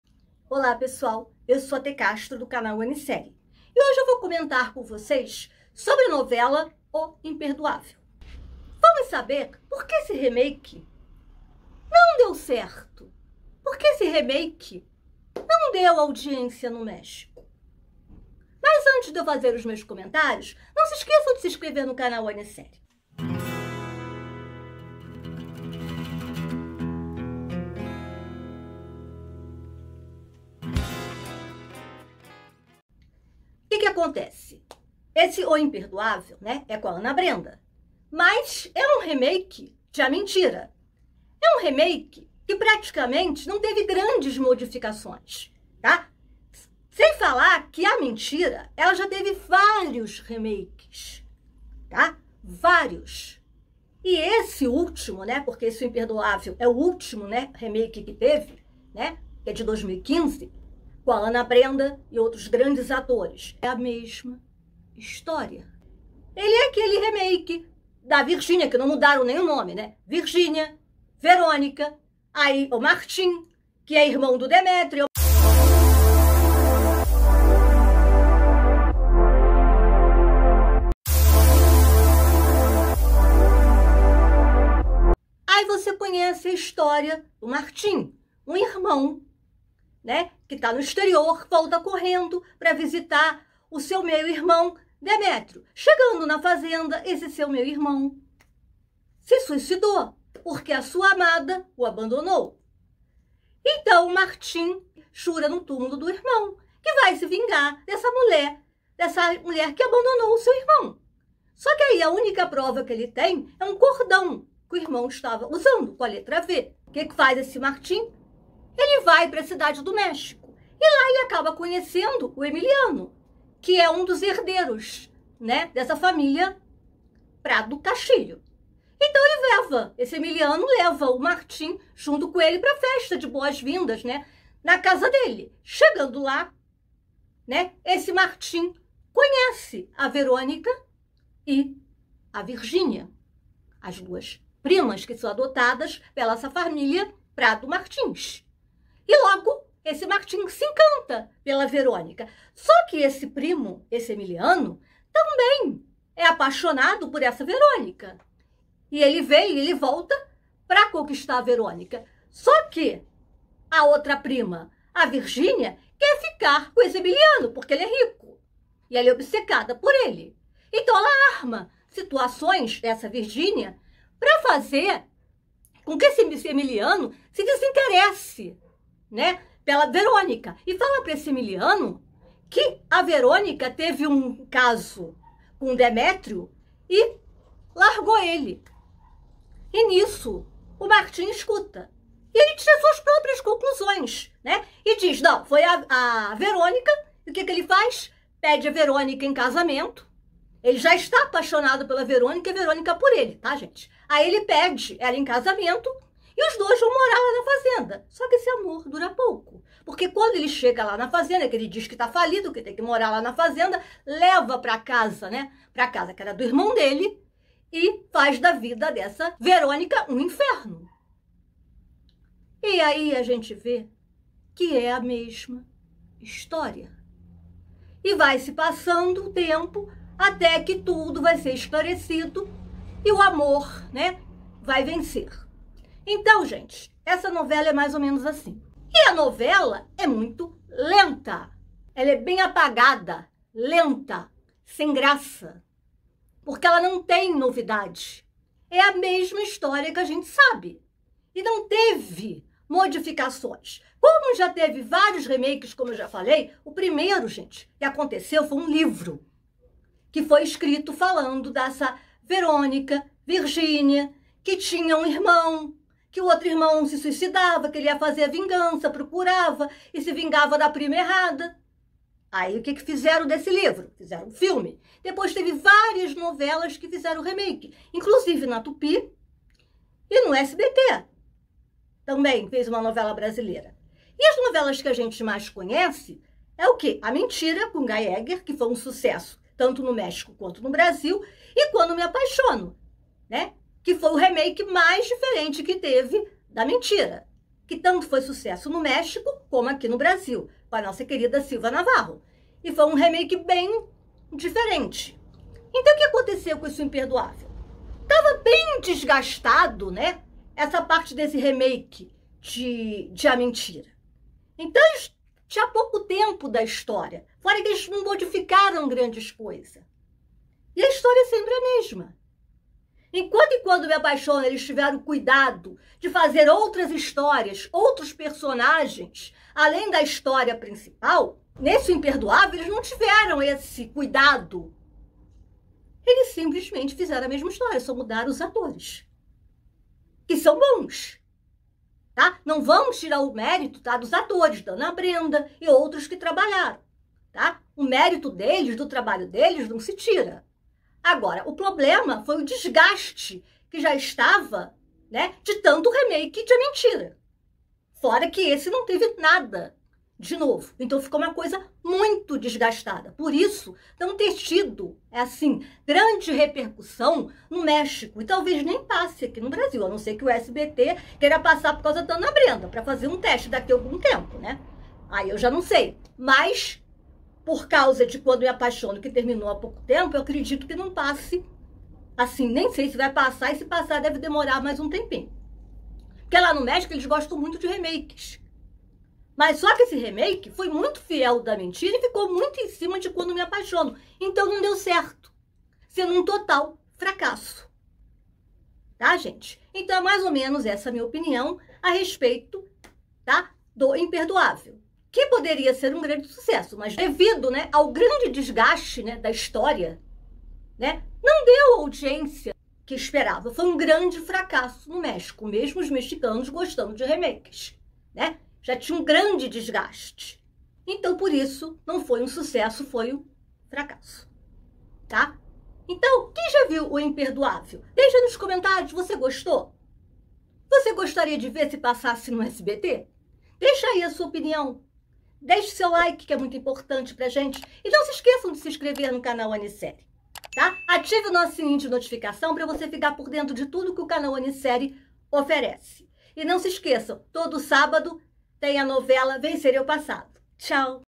Novela O Imperdoável I Por que esse remake não fez tanto sucesso no México?
No vídeo de hoje Te Castro vai falar da novela mexicana Lo Imperdonable (O Imperdoável, no Português) e porque esse remake não teve sucesso no México.

O Imperdoável é um remake da novela A Mentira e que tem no seu elenco Ana Brenda Contreras.

Tem sugestão e indicação de novelas mexicanas, filmes e séries?